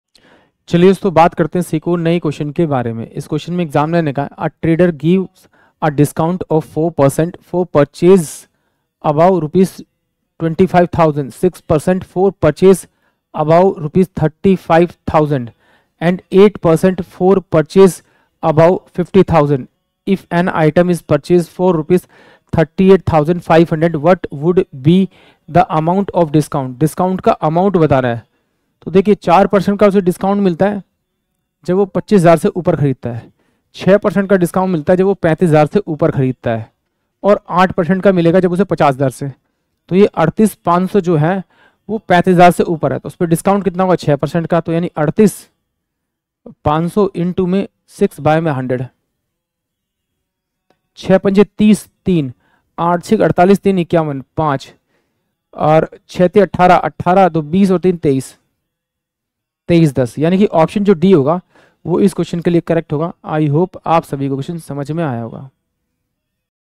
A trader gives a discount of 4 % for purchases above Rs. 25,000, 6%for purchases above
एक व्यापारी 25,000 रुपये से अधिक की खरीद पर 4%, 35000 रुपये से अधिक की खरीद पर 6% तथा 50,000 रुपये से अधिक की खरीद पर 8% की छूट देता है। यदि कोई वस्तु 38,500 रुपये में खरीदी जाती है, तो छूट की राशि क्या होगी?
A trader gives a discount of 4 % for purchases above Rs. 25,000, 6%for purchases above Rs. 35000 and 8% for purchases above 50,000. If an item is purchased for 38,500. what would be the amount of discount?
3,740
1,810
3,080
2,310